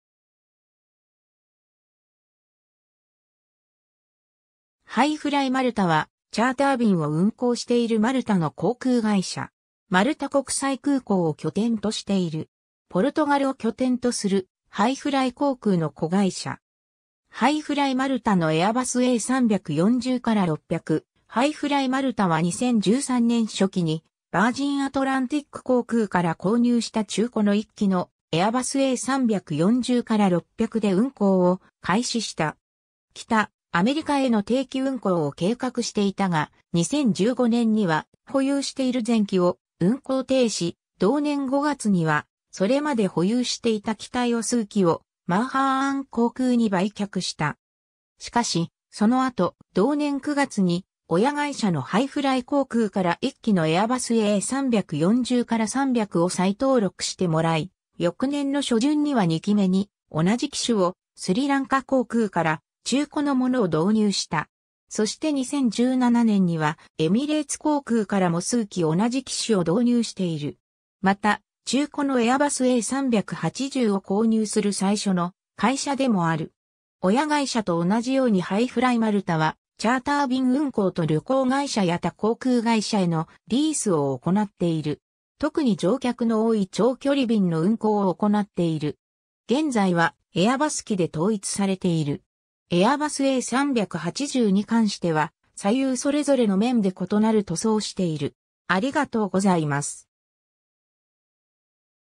ハイフライ・マルタ
ハイフライ・マルタ（Hi Fly Malta）はチャーター便を運行しているマルタの航空会社。
マルタ国際空港を拠点としている。
ポルトガルを拠点とするハイフライ航空の子会社。
 ハイフライ・マルタのエアバスA340-600 ハイフライ・マルタは2013年初期にヴァージンアトランティック航空から購入した中古の1機のエアバスA340-600で運行を開始した。
北アメリカへの定期運行を計画していたが、2015年には保有している全機を運航停止、同年5月には、それまで保有していた機体を数機をマーハーン航空に売却した。
しかし、その後同年9月に親会社のハイフライ航空から一機のエアバスA340-300を再登録してもらい、翌年の初旬には2機目に同じ機種をスリランカ航空から中古のものを導入した。
そして2017年にはエミレーツ航空からも数機同じ機種を導入している。
 また、中古のエアバスA380を購入する最初の会社でも有る。
 親会社と同じようにハイフライ・マルタはチャーター便運行と旅行会社や他航空会社へのリースを行っている。
特に乗客の多い長距離便の運行を行っている。
 現在はエアバス機で統一されている。
エアバスA380に関しては、左右それぞれの面で異なる塗装をしている。